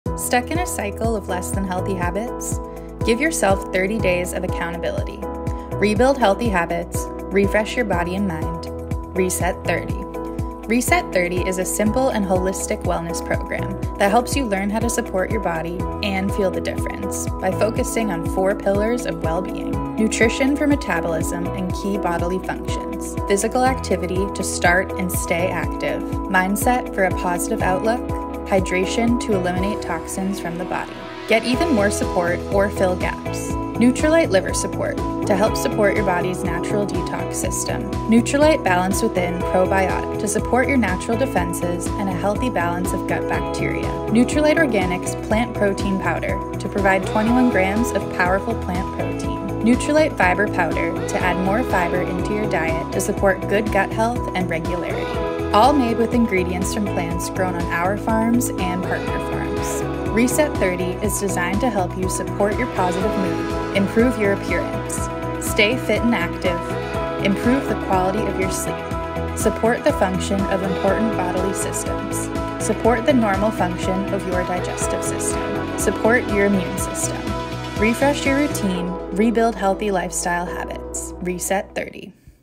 Réinitialisation 30 – Programme de santé et bien-être
Voici Réinitialisation 30, un programme holistique qui facilite la tâche pour retrouver des habitudes saines. Axé sur quatre piliers clés (nutrition, activité physique, état d’esprit et hydratation), cette réinitialisation de 30 jours fournit le soutien dont vous avez besoin pour redémarrer votre routine. Avec des produits Nutrilite, y compris Poudre de protéines végétales, Probiotique Balance Within, Soutien du foie et Fibres en poudre, le programme soutient le processus de nettoyage naturel du corps.